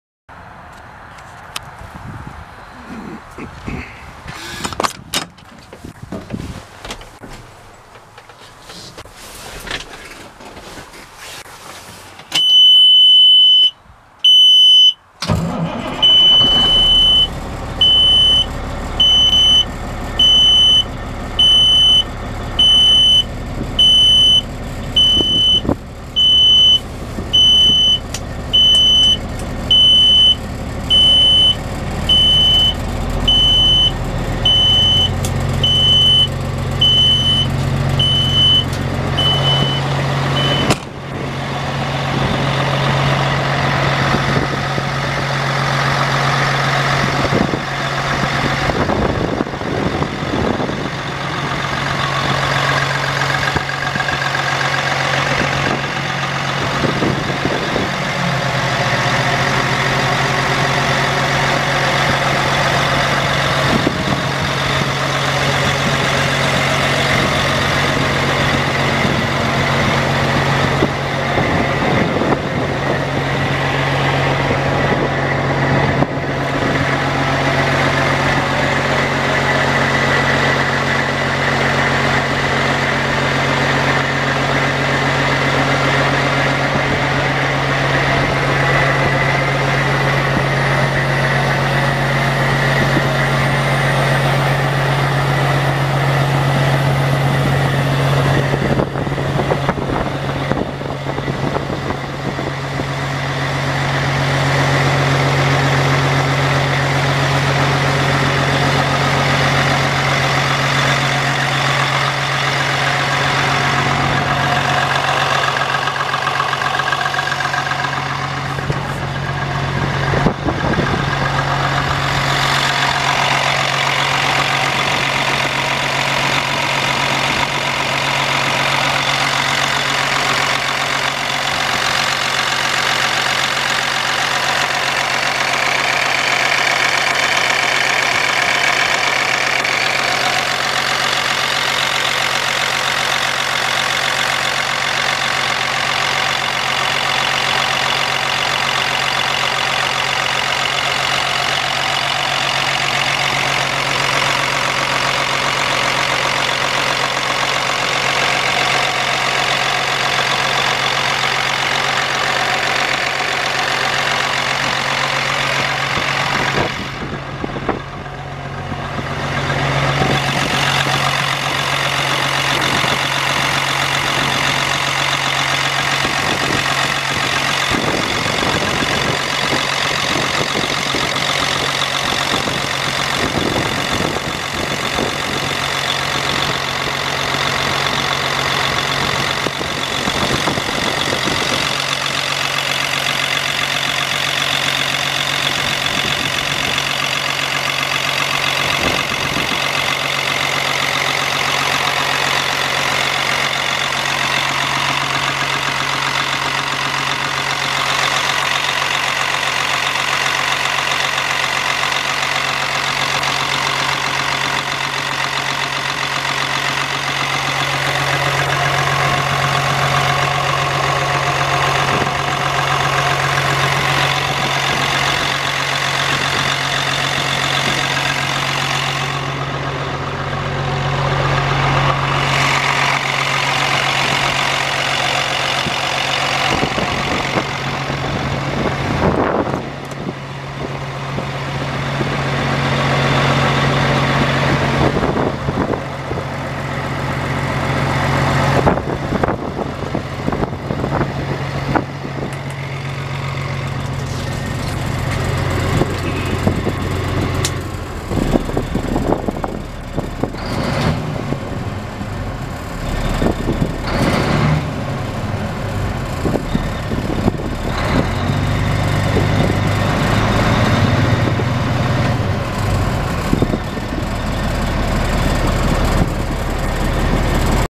1992 Peterbilt 379
Video demonstration of our 1992 Peterbilt 379 from Truck World, Inc.
Wittenberg, WI.